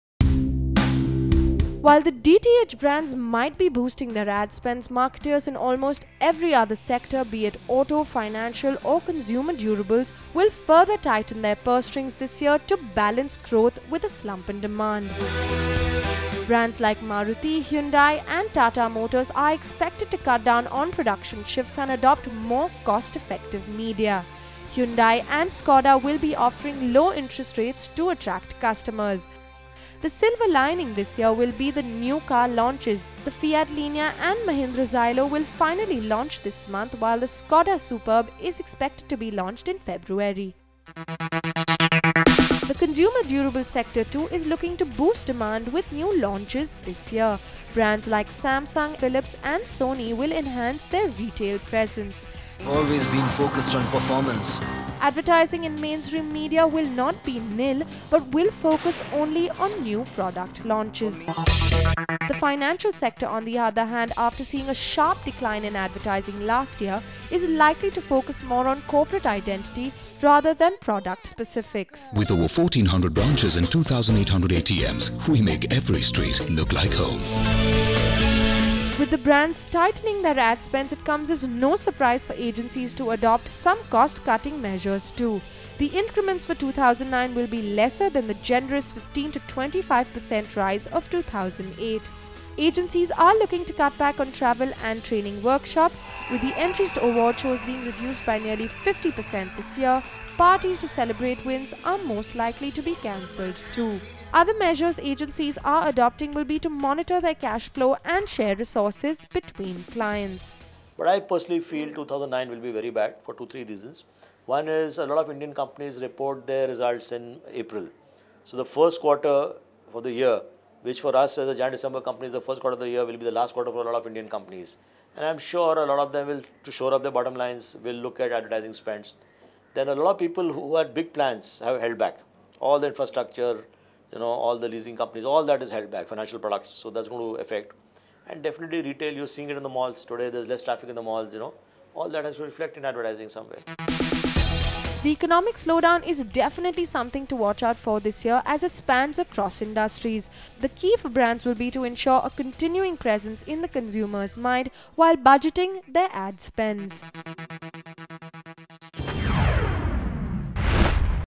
Slow and steady
Marketers in almost every sector.. be it auto, financial or consumer durables will further tighten their purse strings this year to balance growth